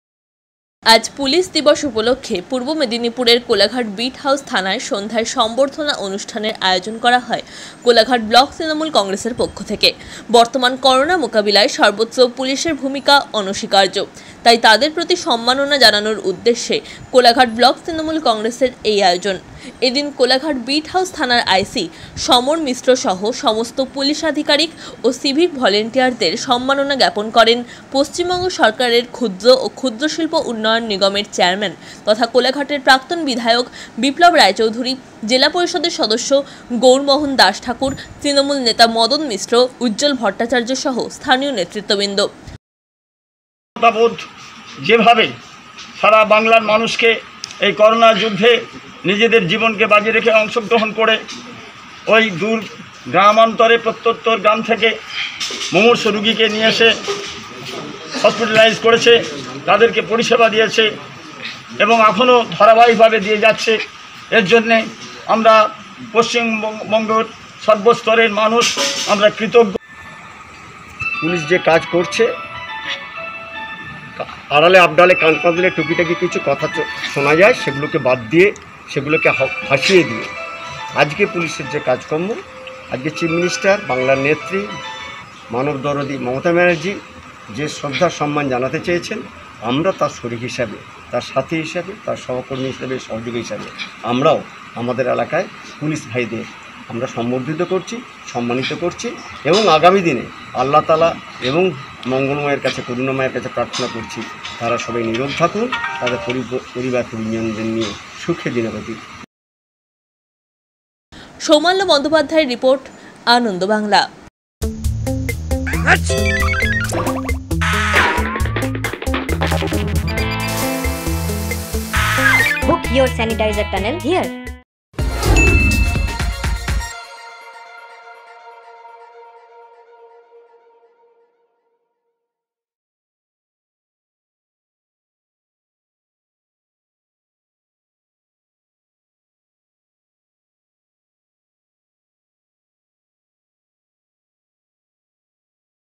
ব্লক তৃণমূল কংগ্রেসের উদ্যোগে পুলিশ দিবস পালন | ANANDA BANGLA | KOLAGHAT
আজ পুলিশ দিবস উপলক্ষে পূর্ব মেদিনীপুরের কোলাঘাট বীট হাউস থানায় সন্ধ্যায় সম্বর্দ্ধনা অনুষ্ঠানের আয়োজন করা হয় কোলাঘাট ব্লক তৃনমূল কংগ্রেসের পক্ষ থেকে।

বর্তমান করোনা মোকাবিলায় সর্বত্র পুলিশের ভূমিকা অনস্বীকার্য। তাই তাদেরপ্রতি সন্মাননা জানানোর উদ্দেশ্যে কোলাঘাট ব্লক তৃনমূল কংগ্রেসের এই আয়োজন।

এদিন কোলাঘাট বীট হাউস থানার আই সি সমর মিশ্র সহ সমস্ত পুলিশ আধিকারিক ও সিভিক ভলেন্টিয়ারদের সন্মাননা জ্ঞাপন করেন পশ্চিমবঙ্গ সরকারের ক্ষুদ্র ও ক্ষুদ্রশিল্প উন্নয়ন নিগমের চেয়ারম্যান তথা কোলাঘাটের প্রাক্তন বিধায়ক বিপ্লব রায়চৌধুরী, জেলাপরিষদের সদস্য গৌরমোহন দাশঠাকুর,তৃনমূল নেতা মদন মিশ্র,উজ্জ্বল ভট্টাচার্য সহ স্থানীয় নেতৃত্ব।